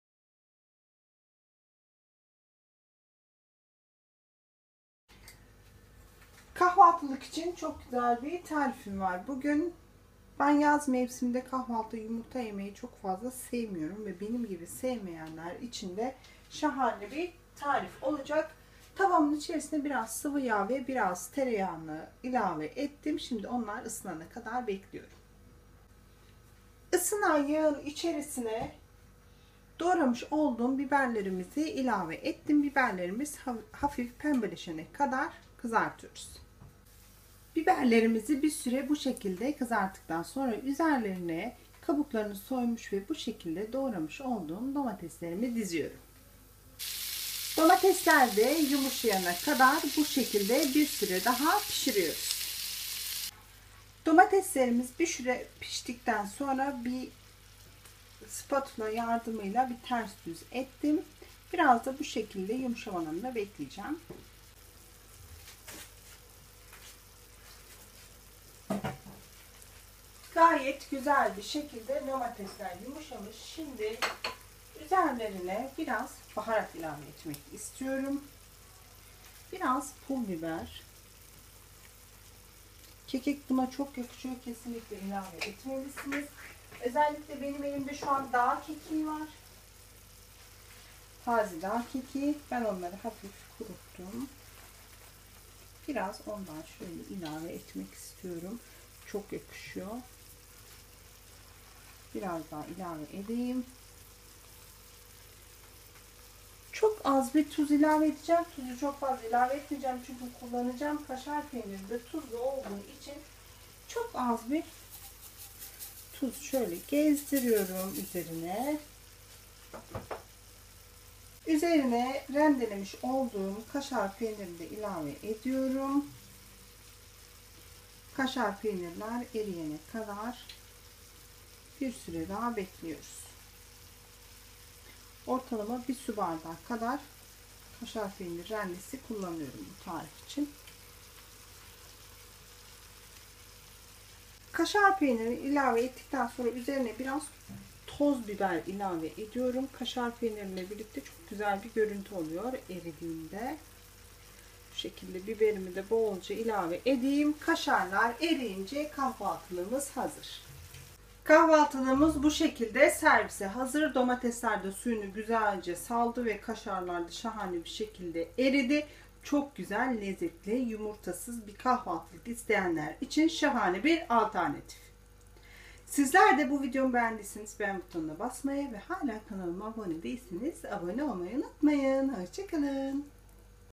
Yumurta Sevmeyenler İçin Yumurtasız Az Malzemeli Kahvaltılık Tarifler
Yaz mevsiminde ben gibi yumurta yemek istemeyenler icin alternatif bi kahvaltilik olsun istedim
biber domates ve kasardan olusan az az malzemeli ve lezzetli bir kahvaltilik